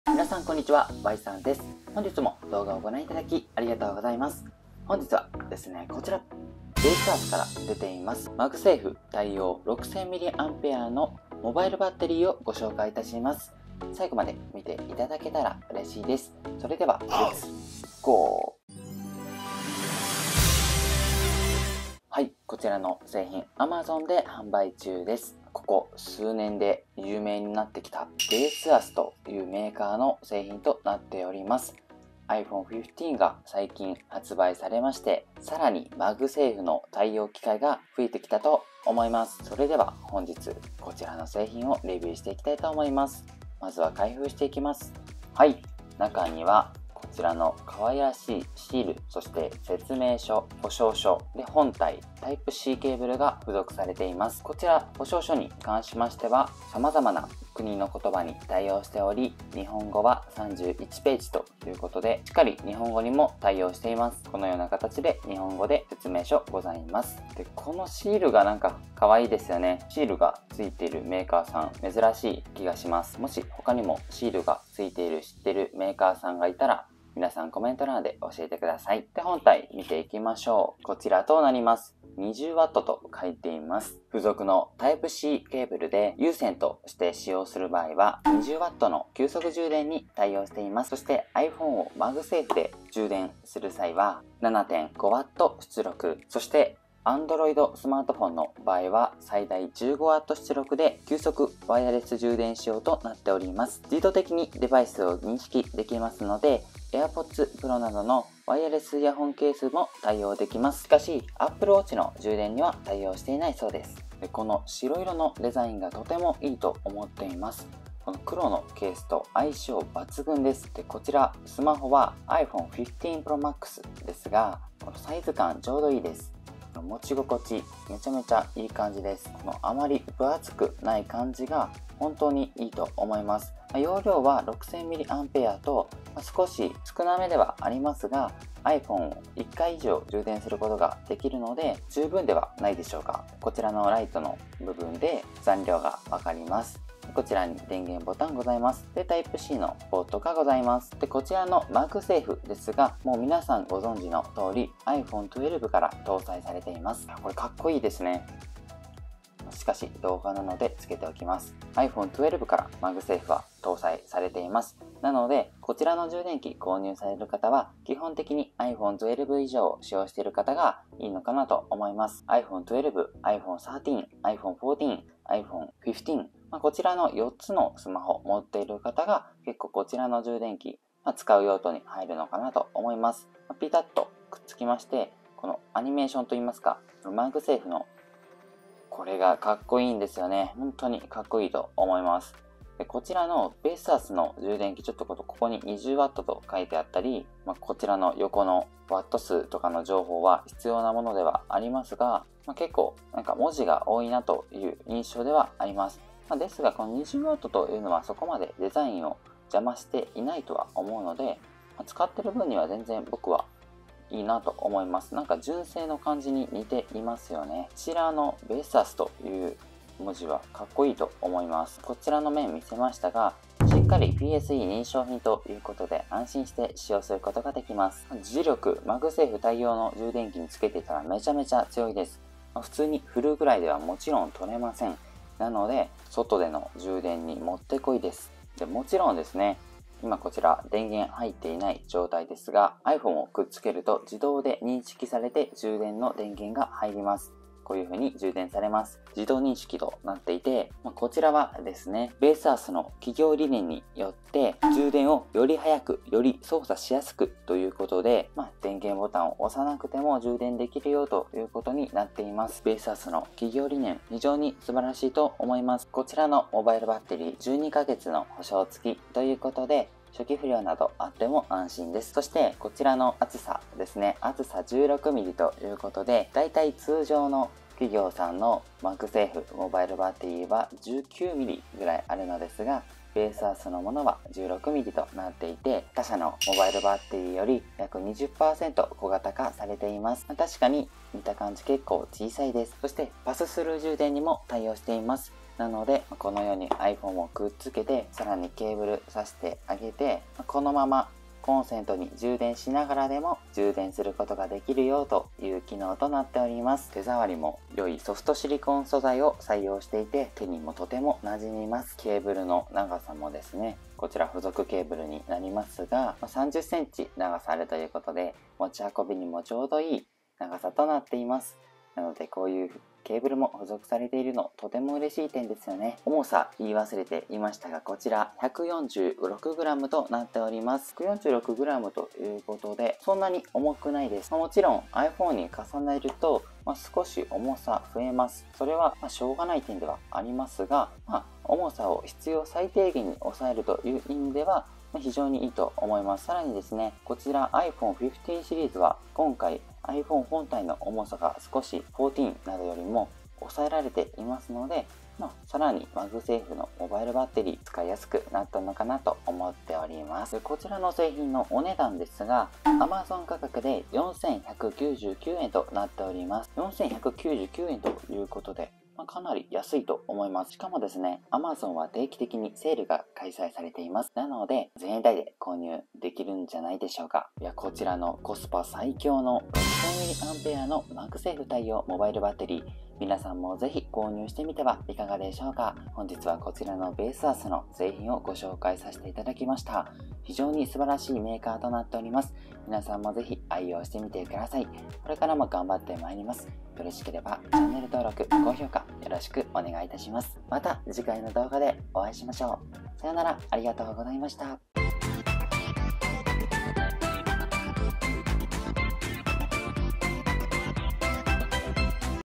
【Baseus】6000mAhで新型iPhoneにピッタリ過ぎるMagSafeモバイルバッテリー！！
提供　Baseus

⬇️動画で紹介した製品の購入はこちらから⬇️

コスパ最強かつ、高性能なMagSafeモバイルバッテリー
見つけちゃいました！！
これあれば1日いや、2日くらいは安心できるかも！（新型iPhoneなら）